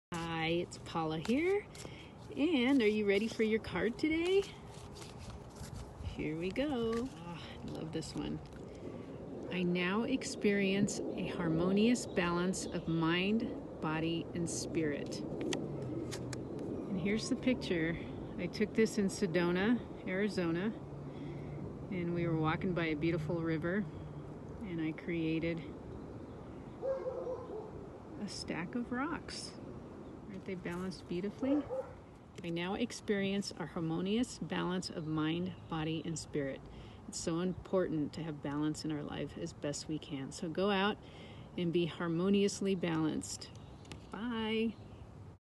Paula's Thought of the Day ep. 2.
Join Paula as she pulls a card from her new grief card deck. Use this affirmation throughout your day to remind yourself of the love and support that surrounds you!
For more information on Paula's Great Loss, Greater Love Card Deck visit our